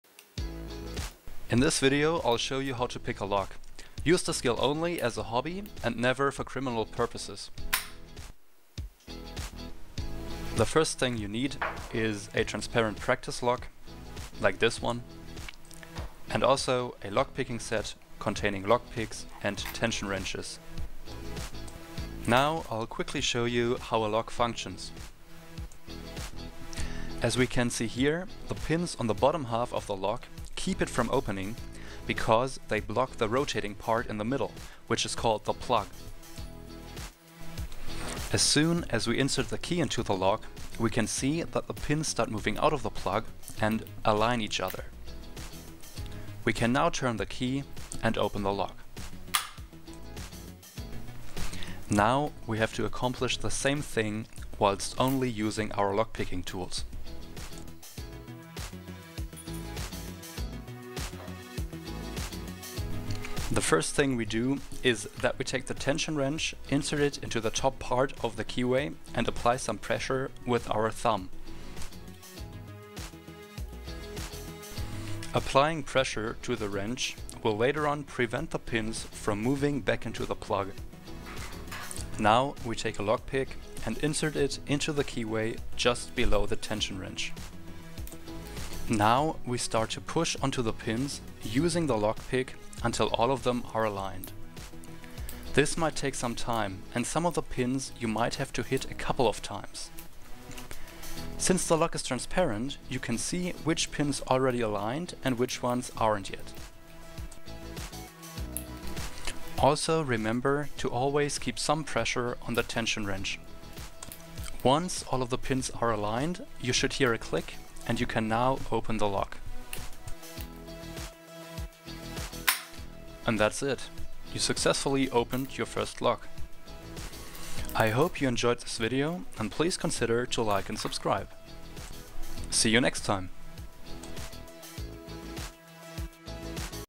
Learn to Pick a Lock in Just Two Minutes - Beginner's Guide
In this video you'll learn how to pick a regular padlock with ease.